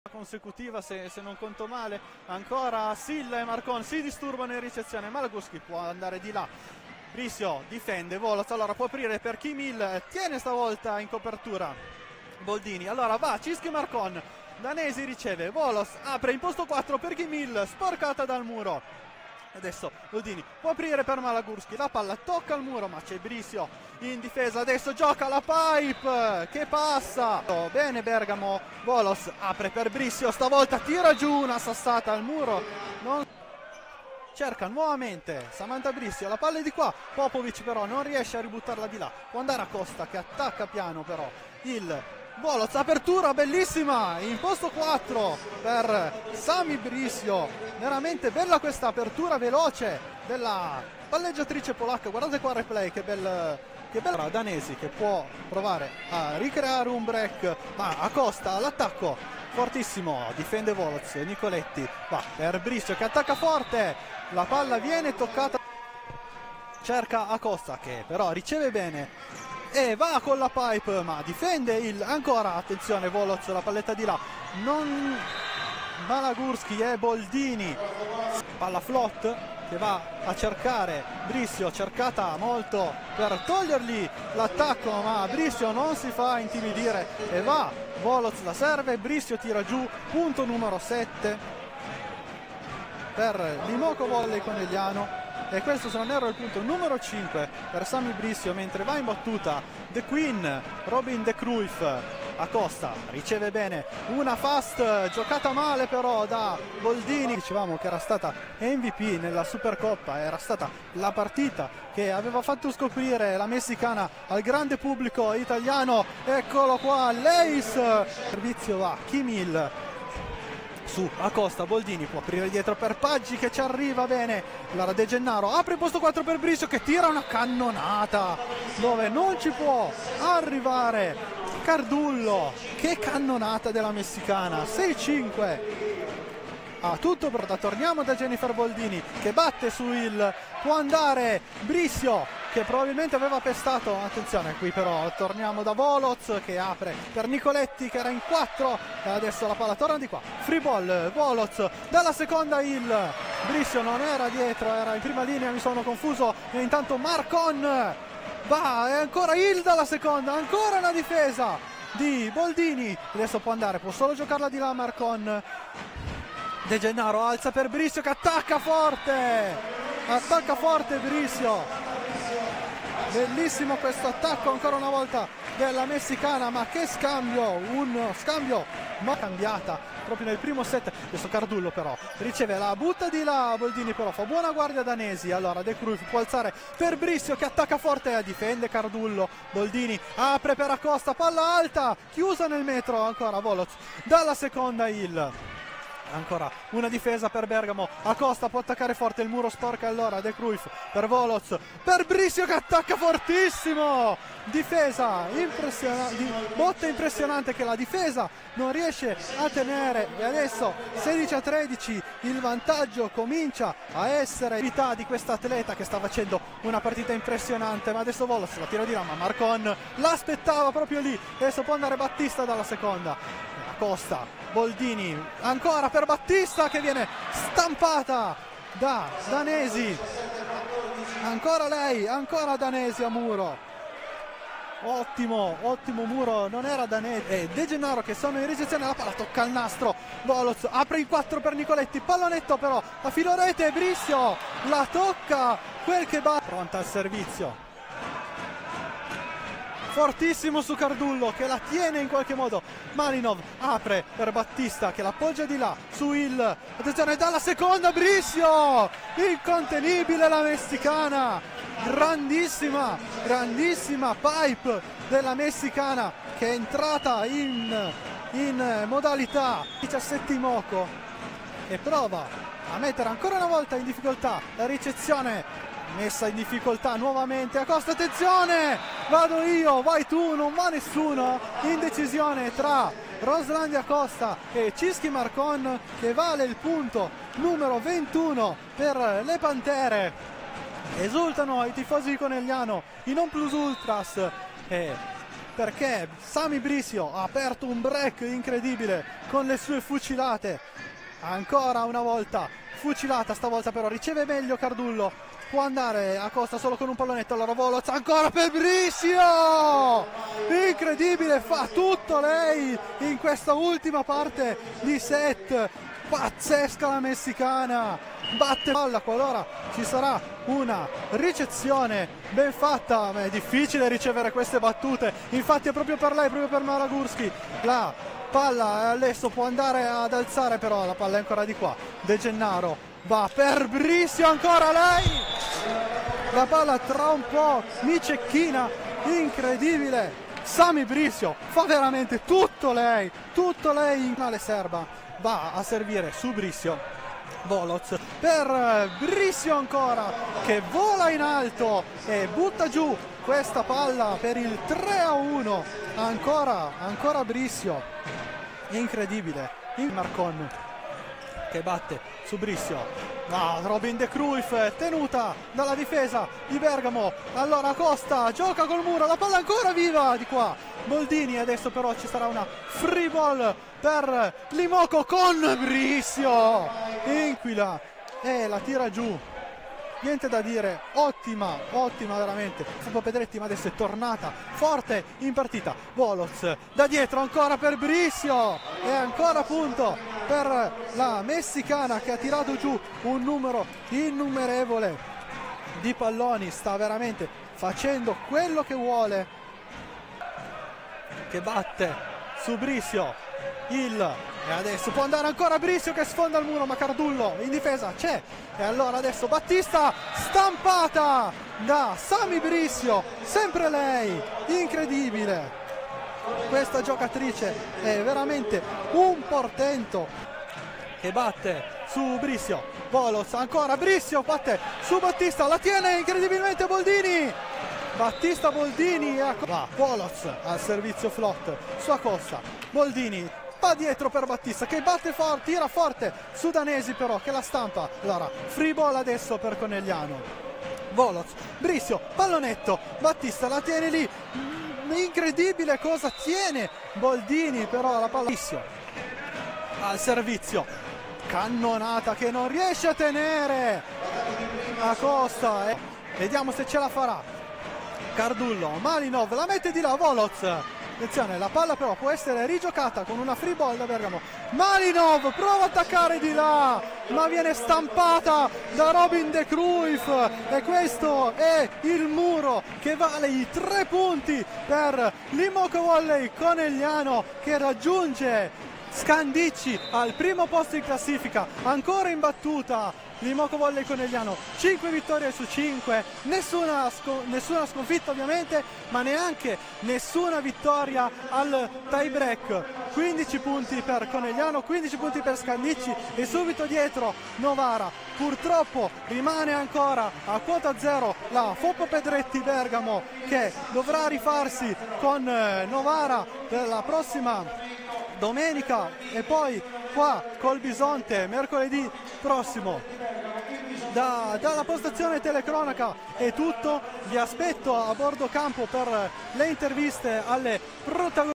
Samantha Bricio (Imoco Volley Conegliano) vs Foppapedretti Bergamo 171115, amazing pipes!
Follow @overthenet2 on IG for more highlights.